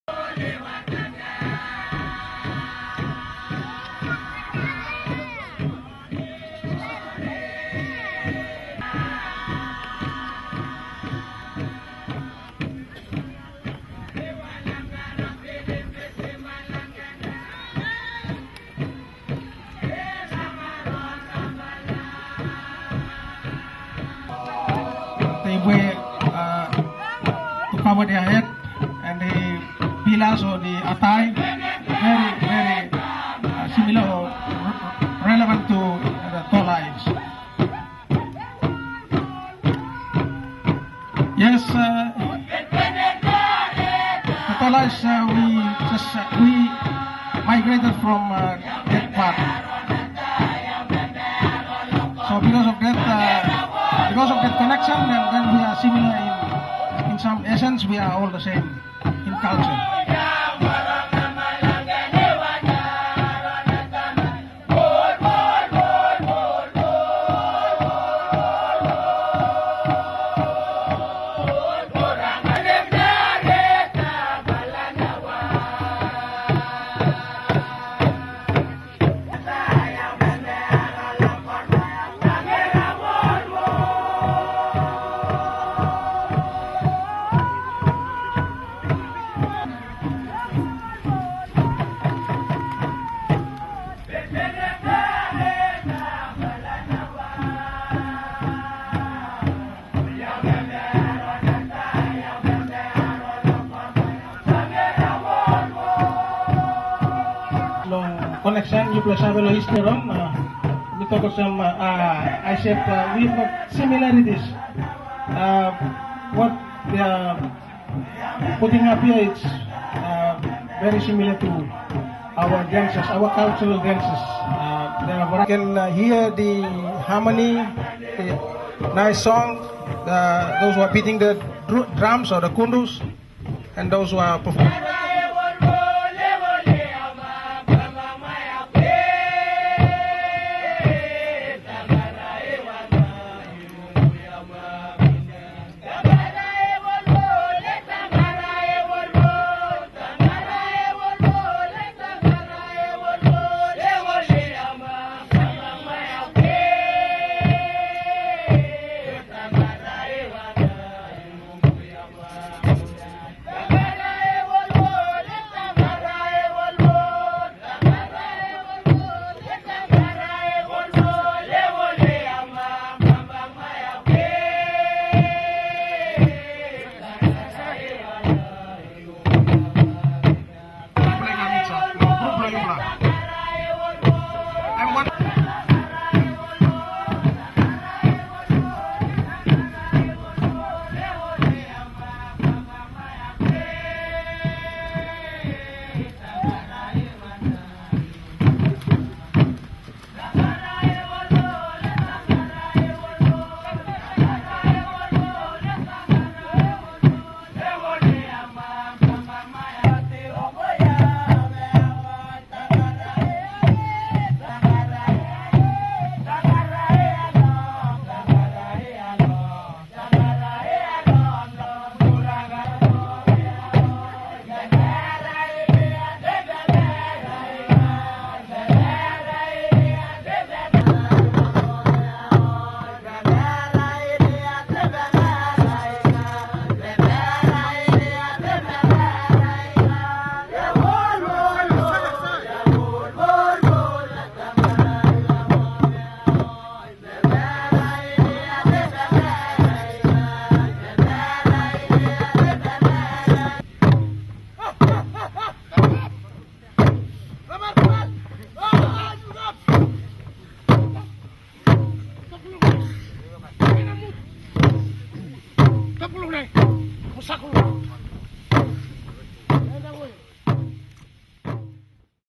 Tangara Dance | Voice of Tribes
Cultural Group from New Ireland performs the Tangara Dance